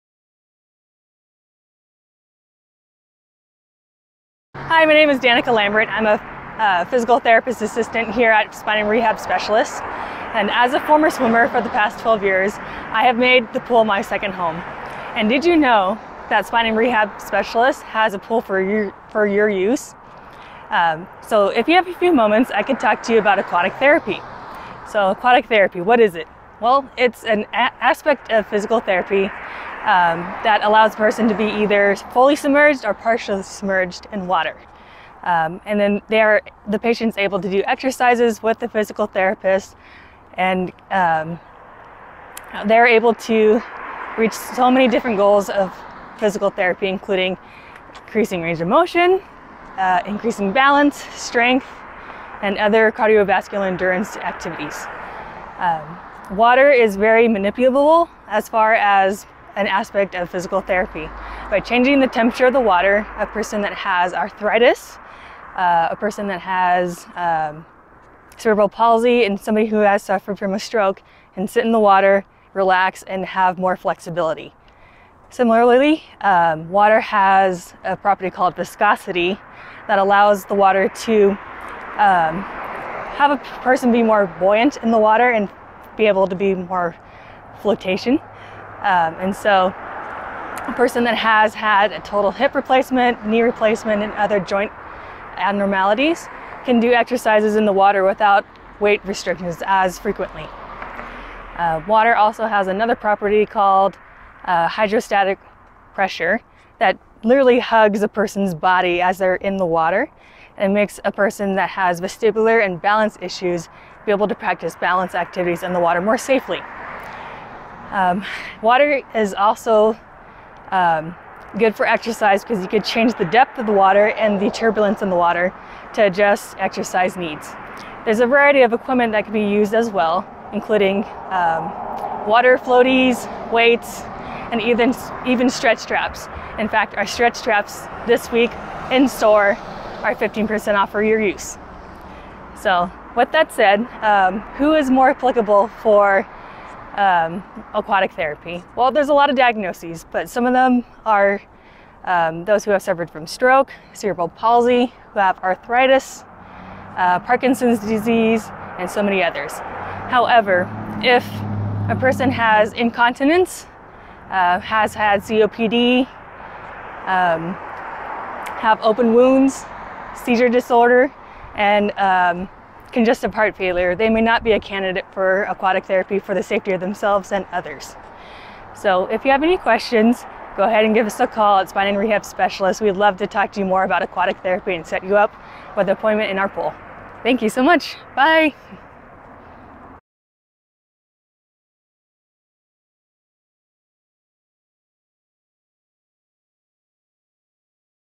Who Can Aquatic Therapy Help? -Sunday Stroll Episode 83-
Walk with Danica this Sunday Stroll as she talks about our in - clinic pool, who is and isn’t eligible for aquatic therapy, her passion for swimming, and how we can help you here at Spine & Rehab Specialists!